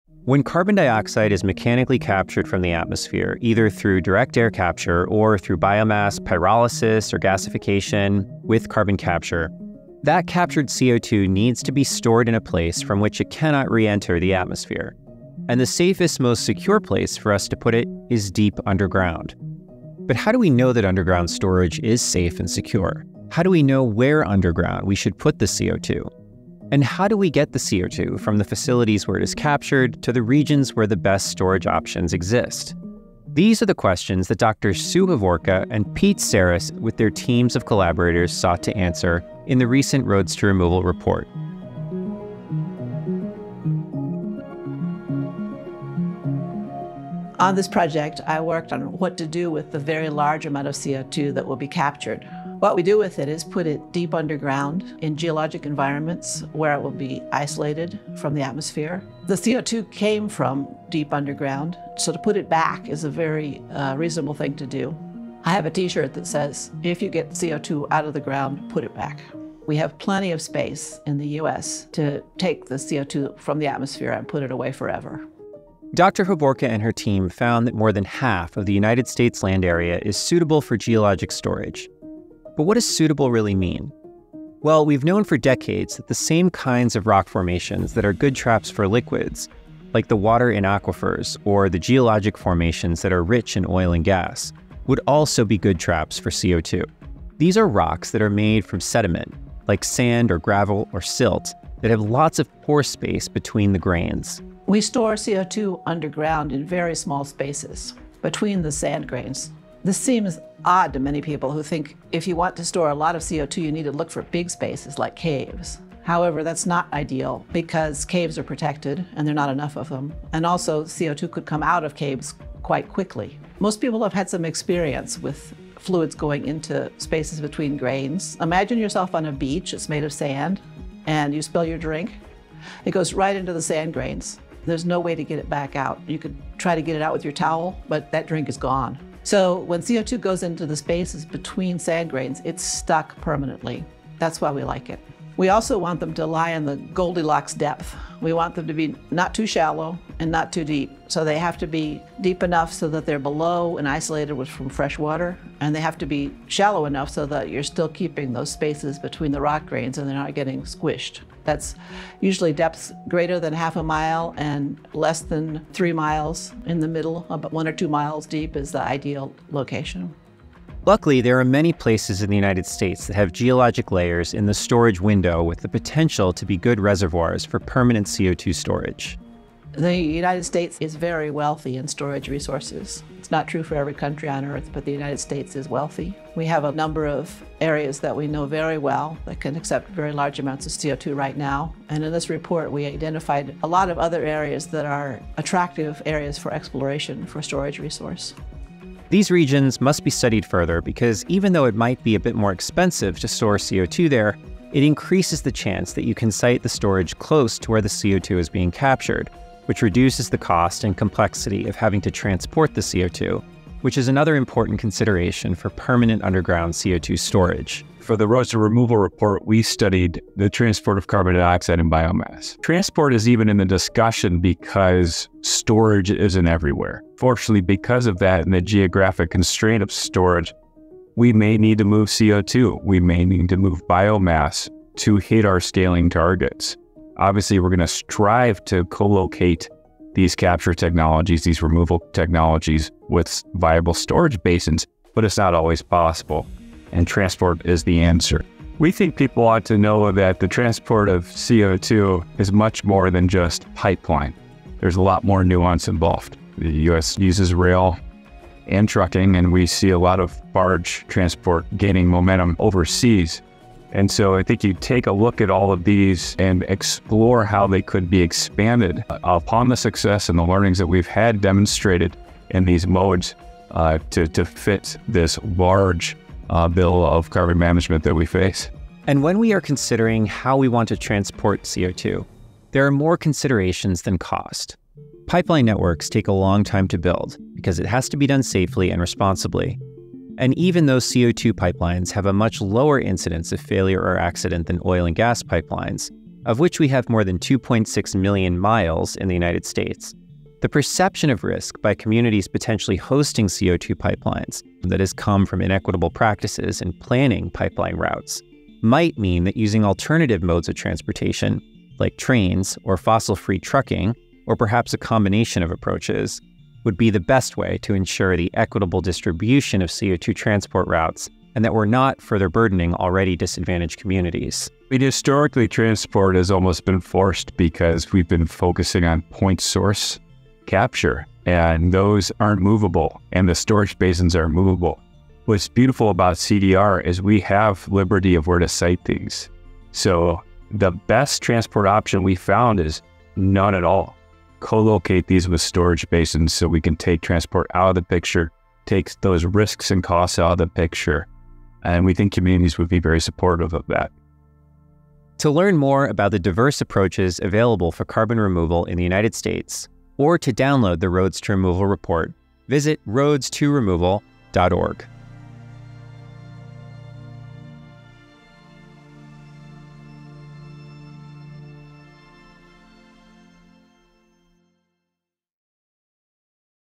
Roads to Removal- Geologic Storage & Transportation
Geologic storage plays a crucial role in removing carbon from the atmosphere, offering a means of long-term storage for CO2. Building on existing comprehensive research on this topic, Roads to Removal researchers conducted a new analysis to determine the distribution and estimated costs of geologic storage resources, and CO2 transportation methods (in order to get CO2 from where it’s captured, to where it can be stored).

The report found that pipelines and barges are the most cost-effective transport methods for regions that will see a lot of CO2 transport, but trucking is the most economical method to transport shorter distances.

The geologic storage analysis includes two new elements. First, it maps the “storage window” where sedimentary rocks are at the right depth to retain CO2 as a dense fluid and enable CO2 injection. Second, it factors in new costs such as how storage volume impacts land-leasing costs, characterization and monitoring expenses, and the benefits to communities. Additionally, the analysis estimates costs on a project basis, allowing developers to find suitable storage options for removal projects based on desired costs.

Over half of the US has potential for CO2 storage, mainly in well-studied areas in the Gulf Coast and dozens of inland basins. Costs vary depending on location and rock formations, with mean storage costs of less than $20/tonne in some areas and higher costs in others.